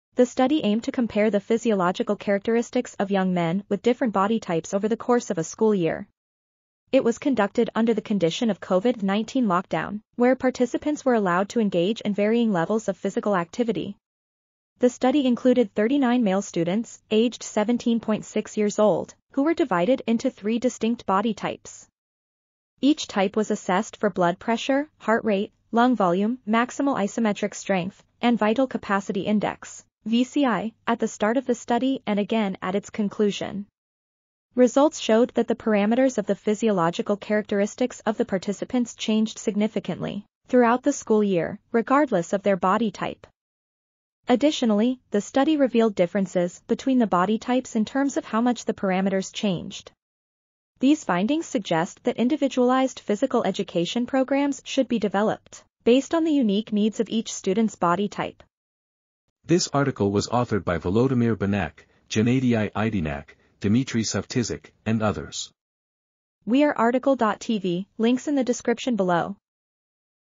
Physiological Characteristics of Young People in the Absence of Mandatory Physical Ac... | RTCL.TV
### Keywords ###

### Article Attribution ###
Title: Physiological Characteristics of Young People in the Absence of Mandatory Physical Activity Required at the University
Authors: Volodymyr Banakh, Gennadii Iedynak, Dmitrii Sovtisik, Lesia Galamanzhuk, Alina Bodnar, Oksana Blavt, Larisa Balatska ,and Oleksandr Alieksieiev
Publisher: OVS LLC
DOI: 10.17309/tmfv.2023.2.14

### Video Timestamps ###
0:00:00 - Summary
0:00:59 - Title
0:01:06 - Outro
0:01:10 - End